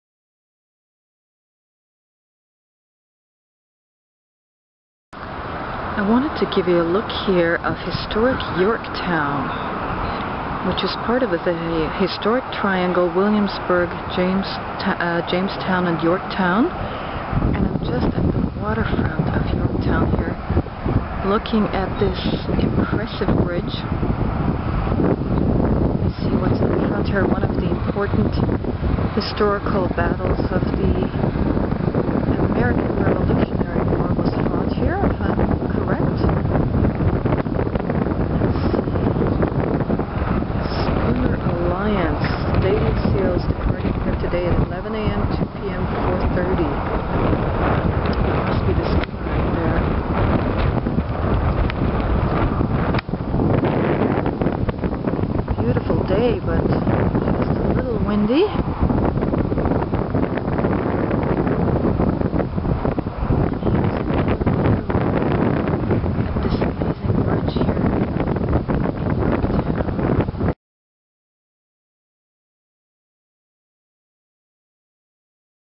Virginia travel: Beautiful waterfront in historic Yorktown
Fall is a great time to travel, so my husband and I decided to book a week in Williamsburg, Virginia from October 18 to 25, 2009. Our home base was the historic Powhatan Plantation from where we did various local excursions. Williamsburg itself is the heart of Colonial Virginia, a city of about 12,000 people that was the capital of the British colony of Virginia from 1699 to 1780. At the time Virginia was Englands oldest and richest colony, particularly thanks to the tobacco plantations that had sprung up

Williamsburg itself is a pretty town set among in rolling verdant terrain and its downtown core features the Colonial Williamsburg Historic Area which recreates 18th century Williamsburg. The idea for this area originated with Reverend Dr. Goodwin of Williamsburgs Bruton Parish Church who was concerned about the survival of Colonial-era buildings and approached billionaire John Dr. Rockefeller Jr. in 1924 to initiate the recreation and restoration of Colonial Williamsburg. Many of the colonial structures that had been torn down earlier were recreated, including the Governors Palace and the Capitol Building. Other important historic buildings include the Bruton Parish Church and the Raleigh Tavern.

Another significant Williamsburg is the College of William and Mary, the second oldest institution of higher education in the United States after Harvard University. This university was founded in 1693 and educated such illustrious personalities as US Presidents Thomas Jefferson, James Monroe and John Tyler. The attractive campus is centered around the Sir Christopher Wren Building, the oldest college building in the US and a National Historic Landmark. I was able to get a nice personal tour of the building which was provided by a very knowledgeable history student from the College.

Part of Virginias Historic Triangle (in addition to Williamsburg) are the towns of Yorktown and Jamestown. Jamestown is the first permanent English settlement in the New World and was established in 1607. Yorktown is the site of the surrender of General Cornwallis to General George Washington in 1781 during the most important battle of the American Revolution. An important battle during the American Civil War (1861 to 1865) also took place here. The Greater Williamsburg area is full of history and will delight anyone with an interest in early American history.

We also made a day trip to Norfolk, Virginias second largest city and an important naval base. Heading further south we stopped in Virginia Beach, a popular beach resort town on the Atlantic Ocean. We played golf at several well-manicured golf courses, all of which were former plantations.  Last but not least, we did a little bit of outlet shopping in Williamsburg, a nice way to cap off a fall vacation.